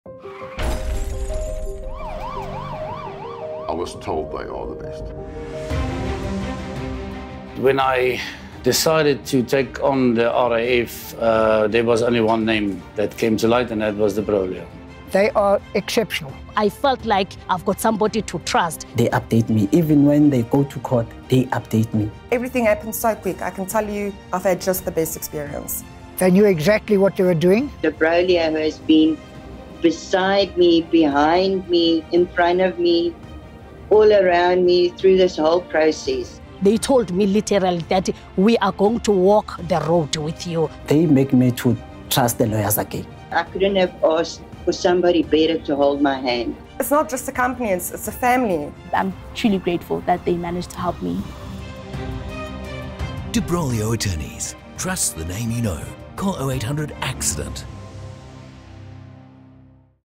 de Broglio 2021 60 second ad
de Broglio Attorneys latest advert - a 60 second ad where clients tell the story - because nobody says it better than clients.
The leaders in Road Accident Fund claims against the RAF.